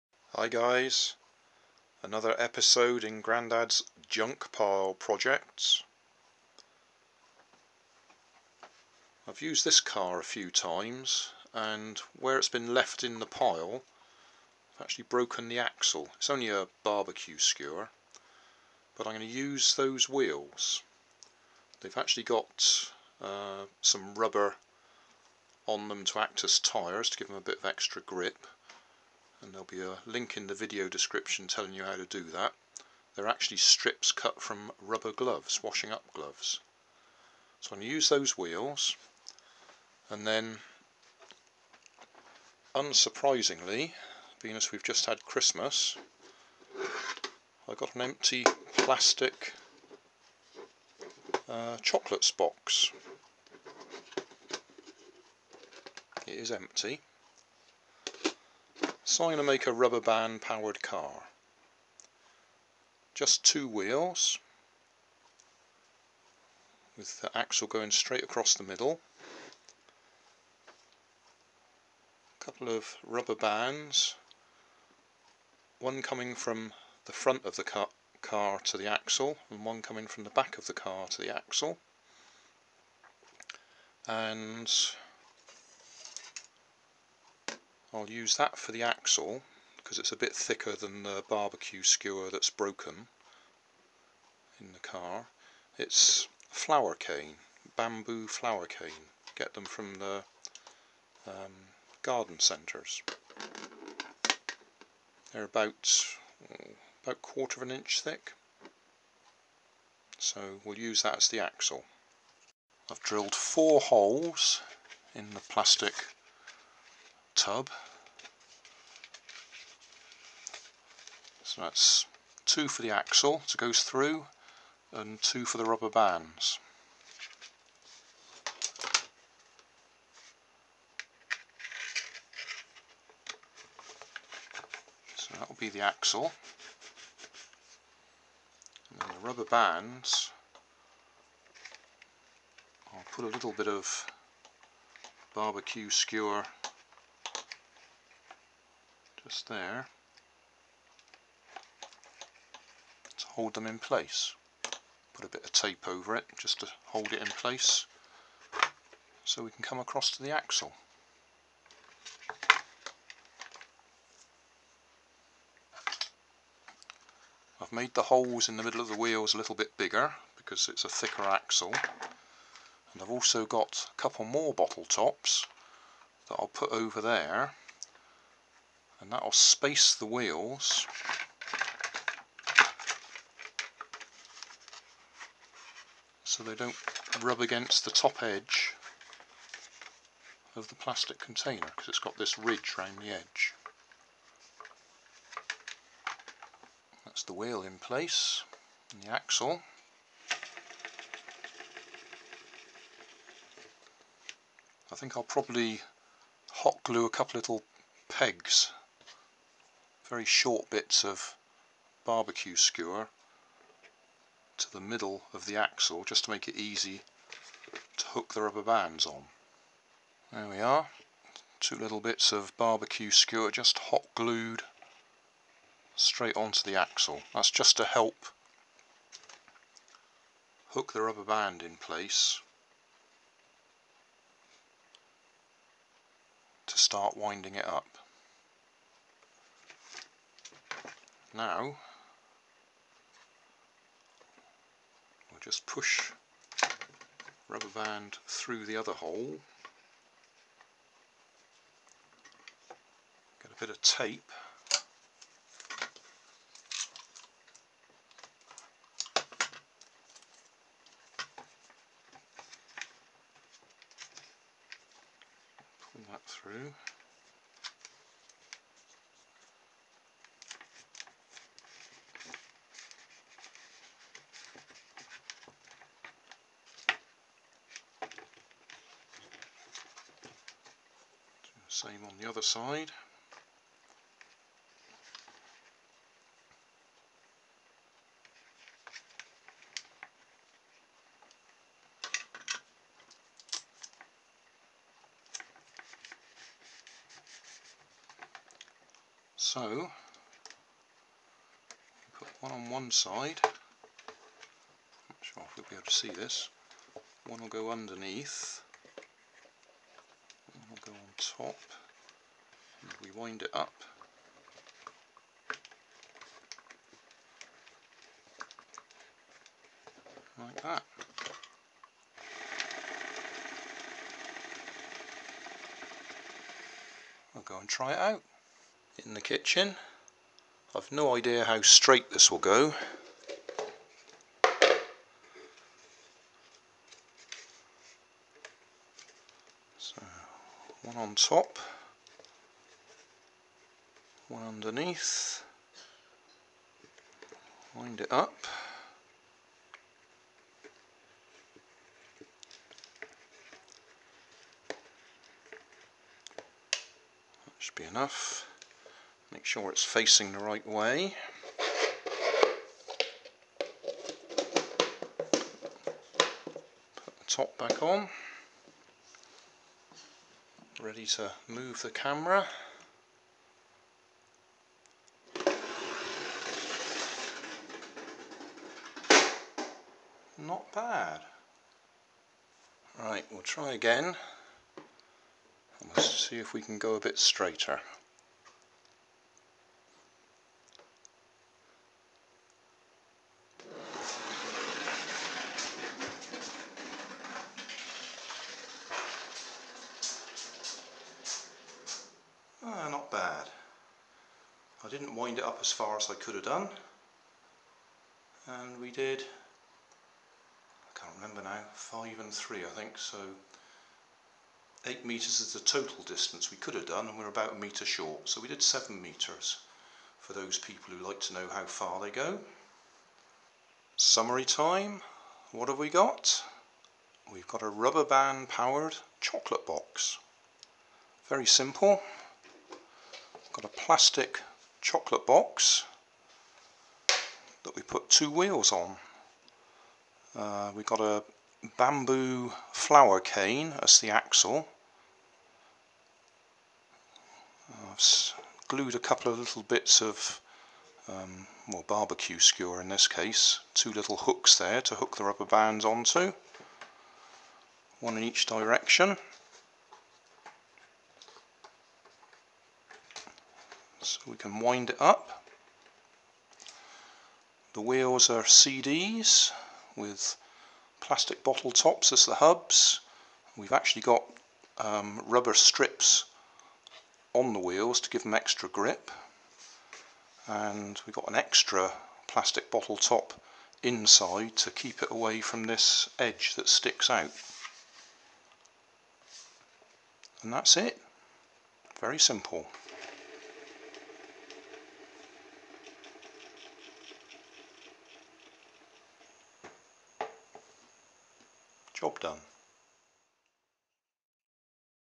Grandad's Junk Pile Projects #10 Chocolate Box Rubber Band Powered Car
Somehow we managed to force ourselves to empty the chocolate box over Christmas and before we throw it out I thought I should make another rubber band powered car.

Main components
Plastic Chocolate Box any similar container would do
2 CDs/DVDs
6 plastic bottle tops
Bamboo flower cane from the garden centre or a similar sized wooden dowel
Rubber bands

Main Tools
Hot Glue Gun
Drill

I have over 2000 videos on YouTube covering a wide range of subjects. To find them follow my search guides